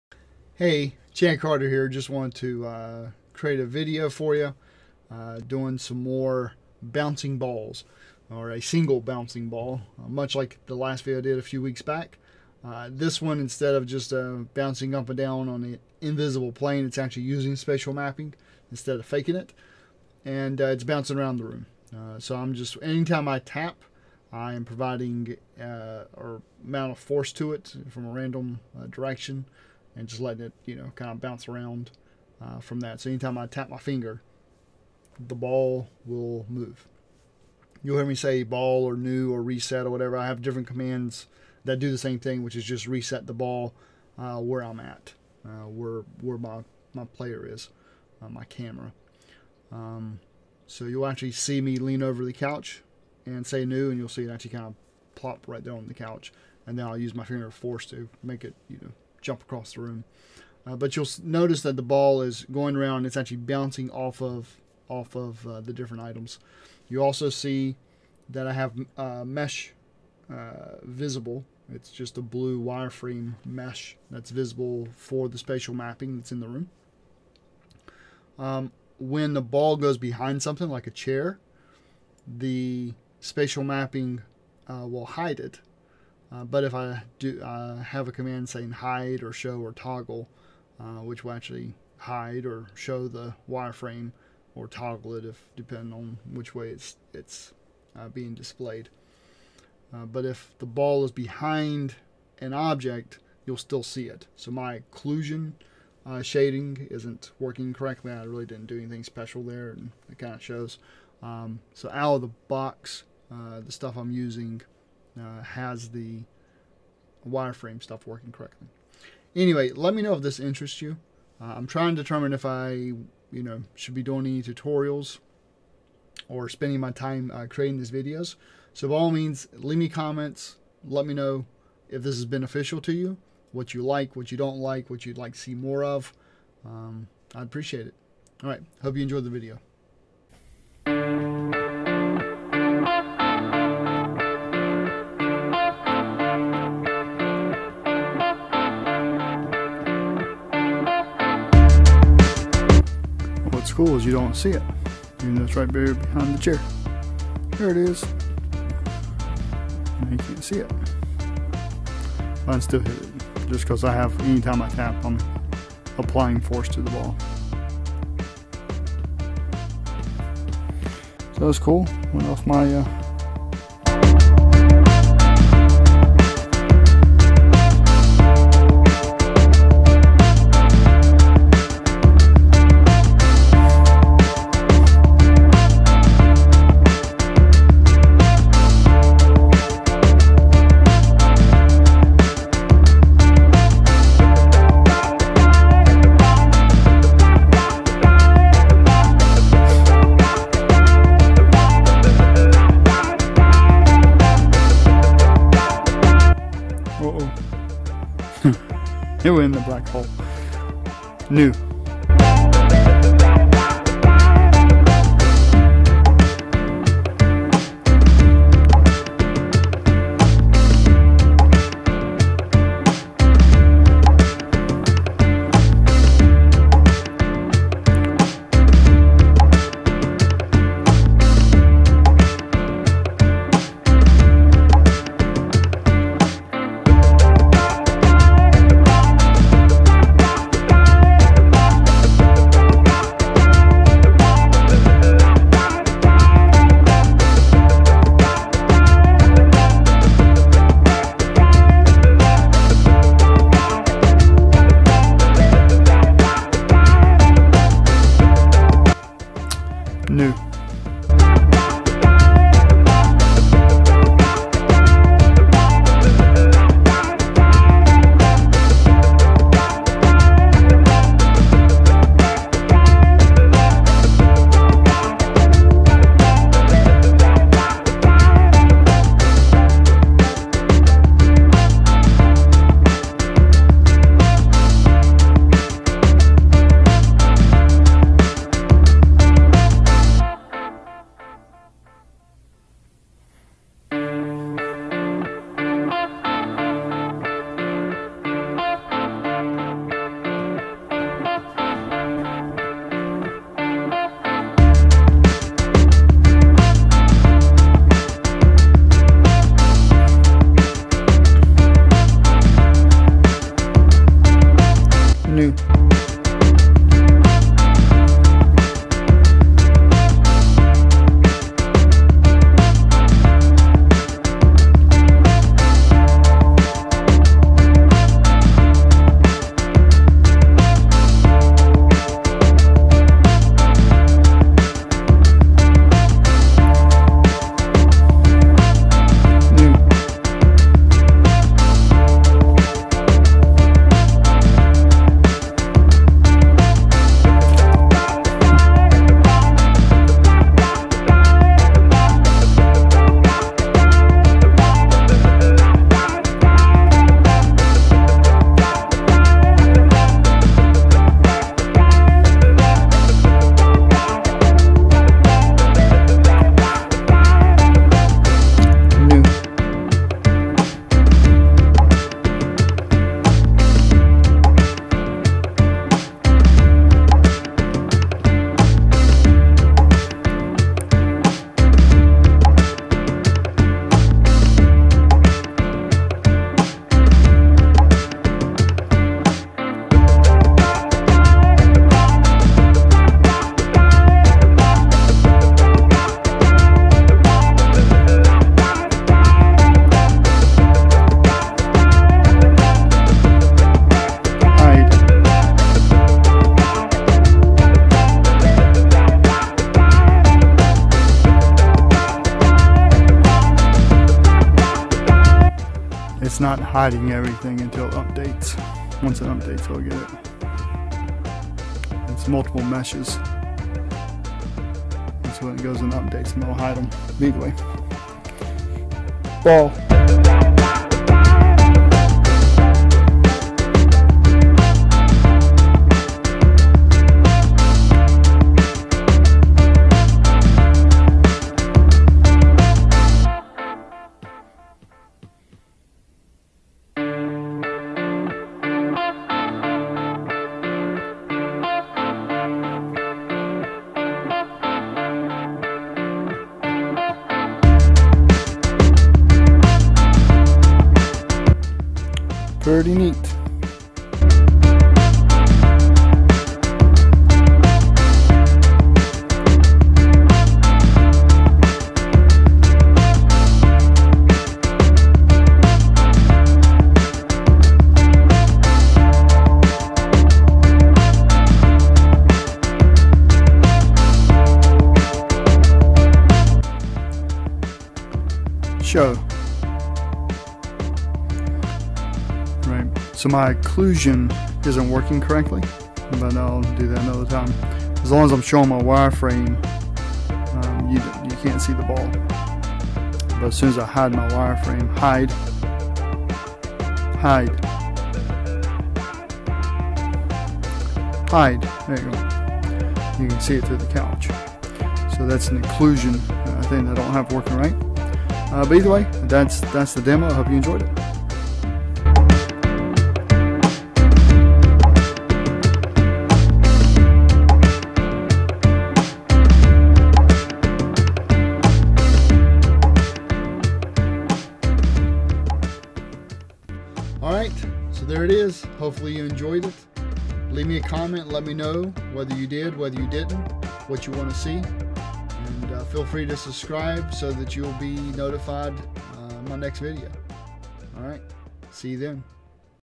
HoloLens Spatial Mapping and a Bouncing Ball
A few weeks ago I created a simple demo of a bouncing holographic ball. I’ve extended the demo to actually take into account the geometry of the room that the HoloLens provides in the form of spatial mapping.

In the demo video below you will the ball bouncing off of the different objects in my basement. The spatial mapping geometry can be seen since I used a blue wireframe material. While the mesh is visible, the ball is hidden when it is behind an object like a couch. It isn’t hidden from view when I hide the spatial mapping data. Once that is hidden, you can see the ball through the couch. I’ll be tackling that at some point in the near future.

If you have anything you want to see let me know. If there is enough interest, I’ll spend some time creating a step by step tutorial.